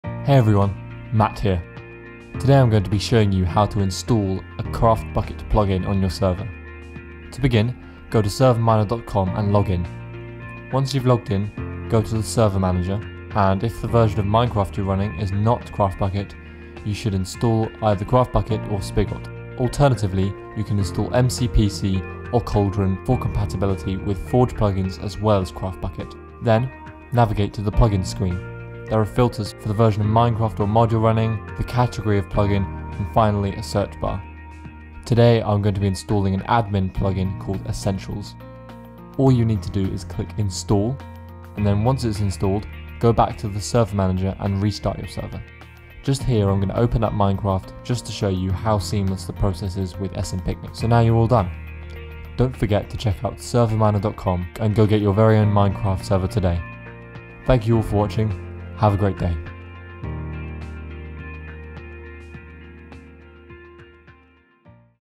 How to Install a Craftbukkit Plugin
This tutorial will show you how to install a Craftbukkit plugin using SMpicnic.